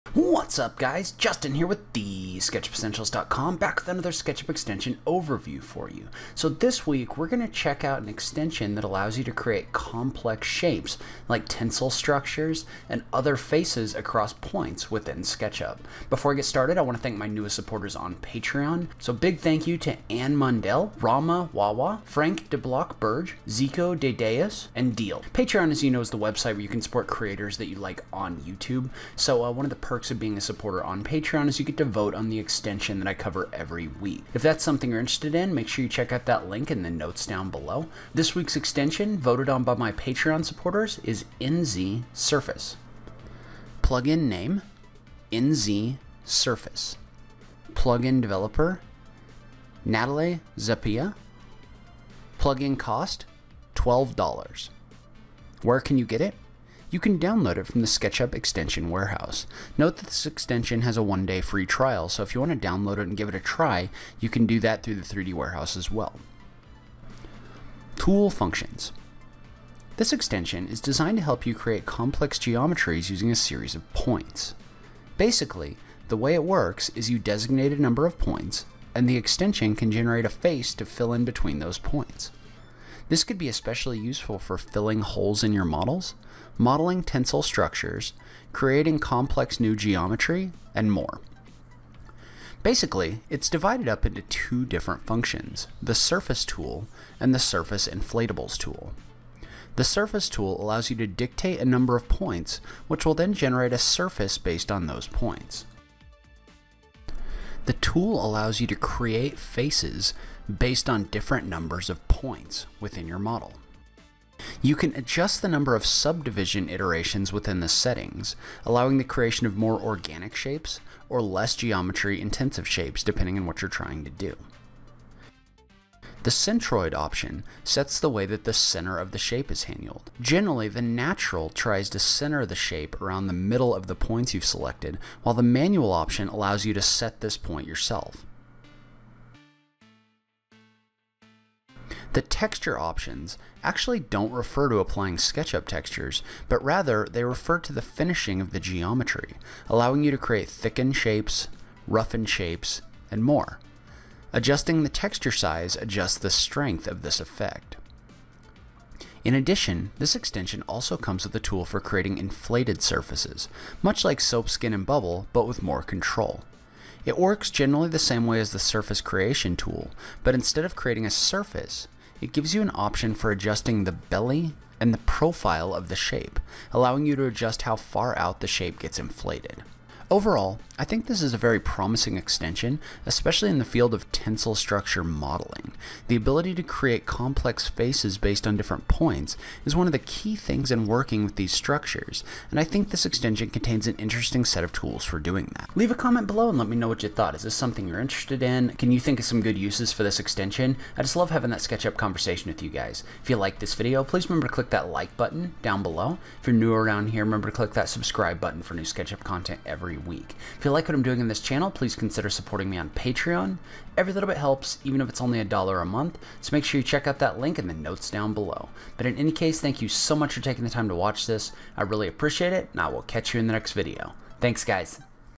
THE BEST TENSILE STRUCTURE EXTENSION for SketchUp? Checking Out nz_Surface!
In today’s video, we’re going to check out an extension that allows you to create complex surfaces within SketchUp!

Plugin Name: nz_Surface
Plugin Developer: Natale Zappia
Plugin Cost - $12
Where can you get it? You can download it from the SketchUp Extension Warehouse.
Tool Functions –
This extension is designed to help you create complex geometries using a series of points. Basically, the way it works is that you designate a number of points, and the extension can generate a face to fill in between those points.
This could be especially useful for filling holes in your models, modeling tensile structures, creates complex new organic geometry, and more.
Basically, it’s divided up into two different functions – the surface tool, and the surface inflatables tool.
The surface tool allows you to dictate a number of points, which will then generate a surface based on those points.
You can adjust the number of subdivision iterations within the settings, allowing the creation of more organic shapes, or less geometry intensive shapes.
The centroid sets the way that the center of the shape is handled – generally, the natural seems to center the shape around the middle of the point, while the manual option allows you to place the center point.
The texture options don’t actually refer to applying SketchUp textures, but instead refer to the finishing of the geometry, allowing you to create thickened shapes, roughened shapes, etc. The texture size will adjust the strength of the effect.
In addition, this extension also comes with a tool for creating inflated surfaces – much like soap skin and bubble, but with more control. It works generally the same way as the surface creation tool, but instead of creating a surface, it gives you an option for adjusting the belly and the profile of the shape, allowing you to adjust how far out the shape is inflated.
Overall, I think this is a very promising extension, especially in the field of tensile structure modeling. The ability to create faces based on points is one of the key things in working with these structures, and I think this extension contains an interesting set of tools for doing that.
It comes with a short trial (I believe 1 day), so give it a try and let me know what you think!